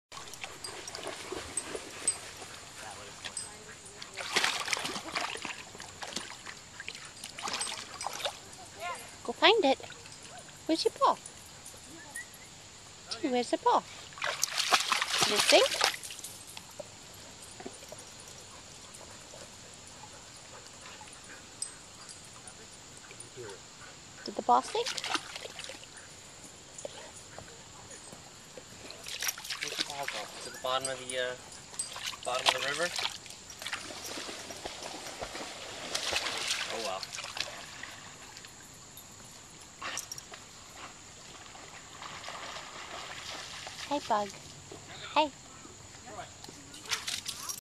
Doberman's ball sinks in Pond
Our Doberman, troy took his ball down into the pond and lost it. Here he is looking for it.